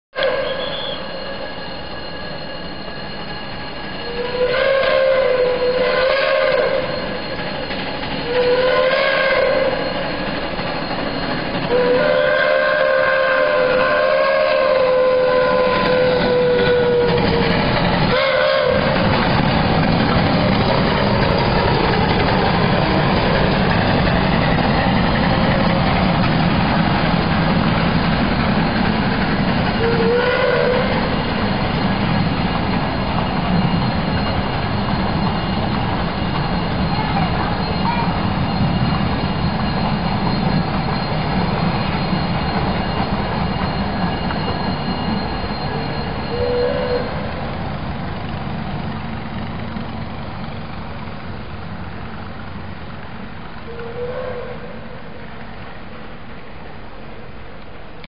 Tren Turístico de la Sabana
Vestido para la Navidad, pasando detrás de mi casa.